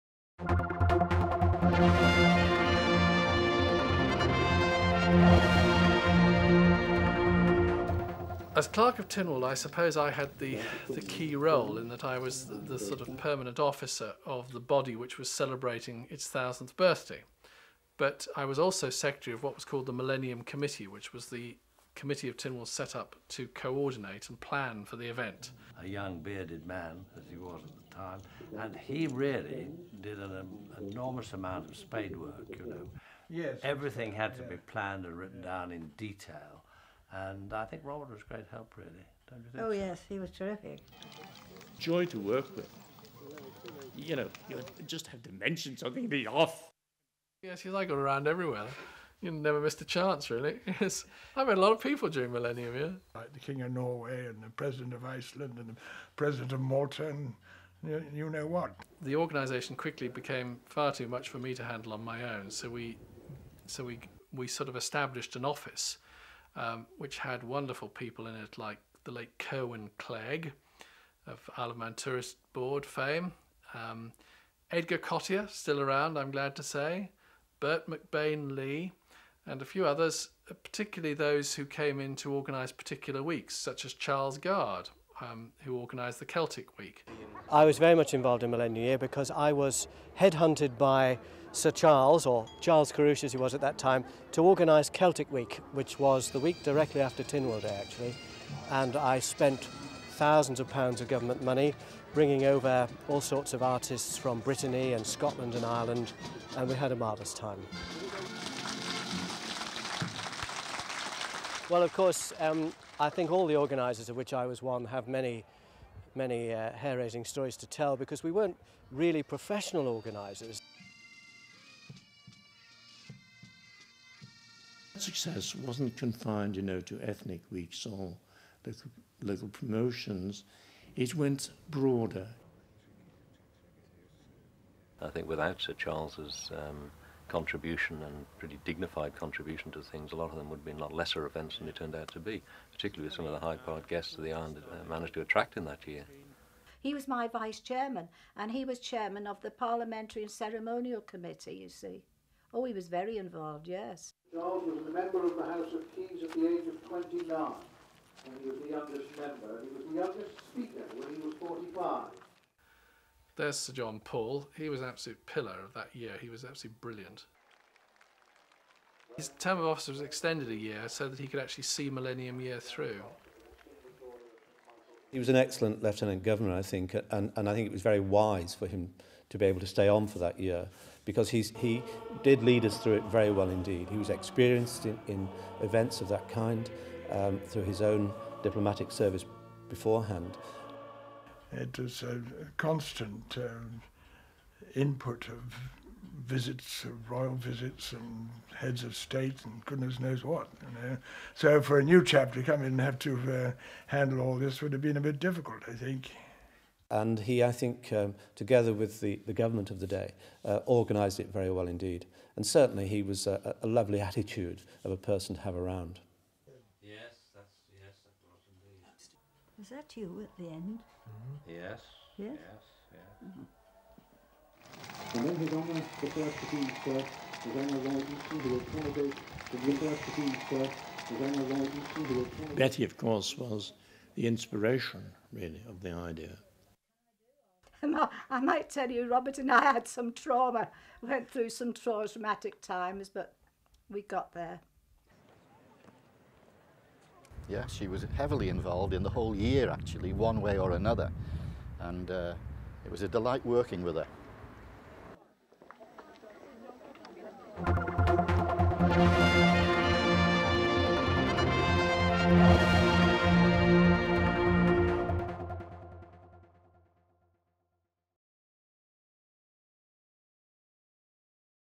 IoM TV archive: Manx Millennium 1979 (6)
The organisers of the Manx Millennium talk about what it was like to plan the event. With contributions from former clerk of Tynwald Robert Quayle, ADC to Lieutenant Governor Lt Col Brian Mylchreest, Speaker of the House of Keys Sir Charles Kerruish, Lieutenant Governor Sir John Paul, Manx Nationalist Charles Guard, Manx Radio station manager Stewart Watterson, Archdeacon of Man Brian Partington and former headteacher Queen Elizabeth II High School.